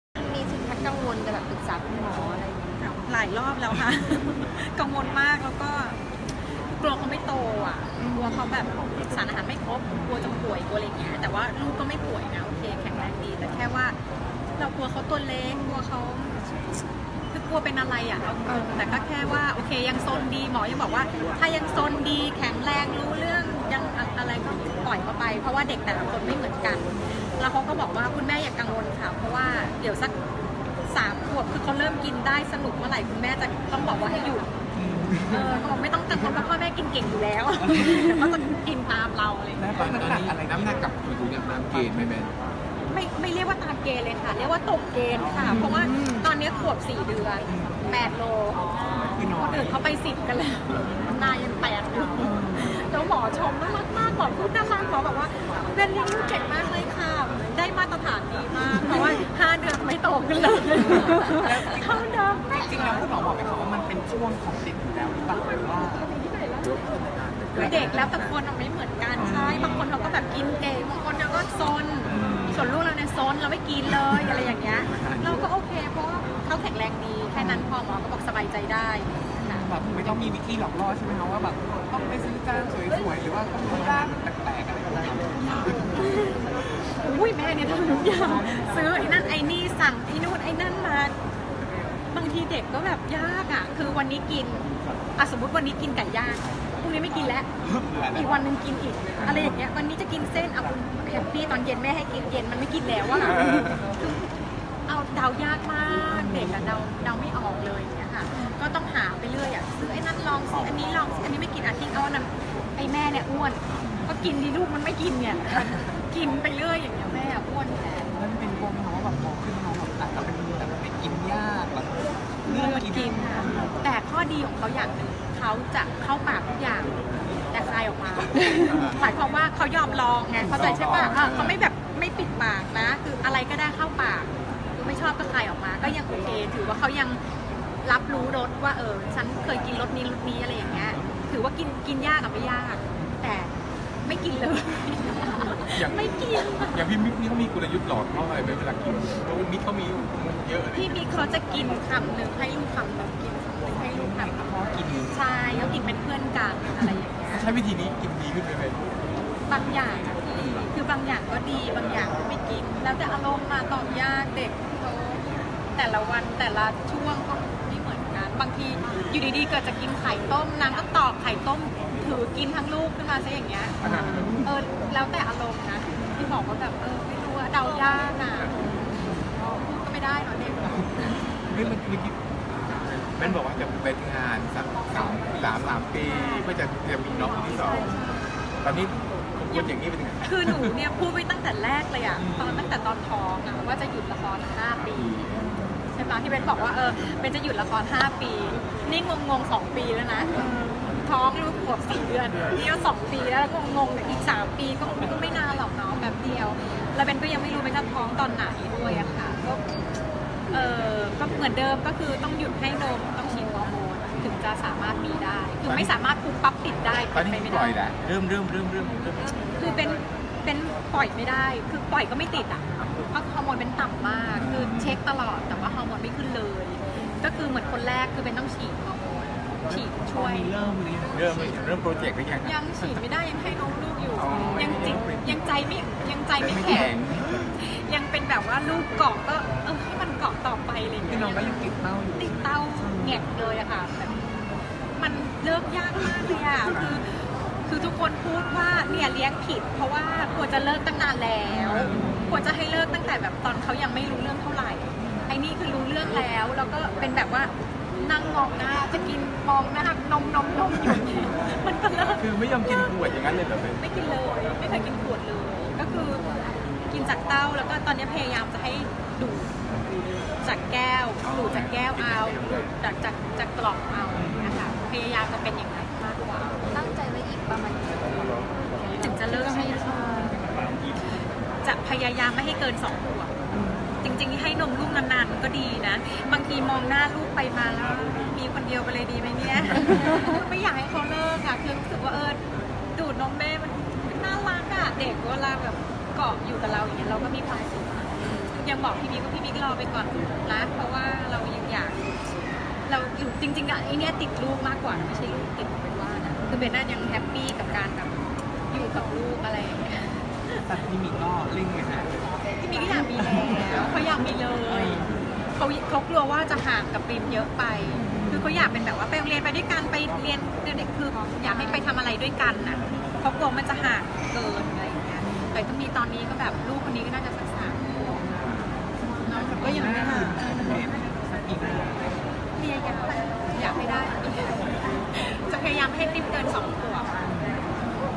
เบนซ์ พรชิตา จับมือสามี เดินหน้าทำสิ่งนี้ ปีหน้ามีเฮ!!
เบนซ์ พรชิตา ห่วงน้องปริมไม่ค่อยทานอาหาร หวั่นลูกตัวเล็กต่ำเกณฑ์ หรือป่วย เผยปรึกษาหมอพัฒนาการปกติ ไม่มีปัญหา แต่ส่วนตัวกลัวพอโตขึ้นจะเป็นเด็กกินยาก บอกมิกค์ สามี ช่วยหลอกล่อเวลาป้อนข้าว ตั้งแต่รอน้องปริมหย่านม ฉีดฮอร์โมน เตรียมปั้มลูกคนที่2ทันที หวังอยากให้โตทันกัน ลุ้นถ้าเป็นไปได้อยากมีปีหน้าเลย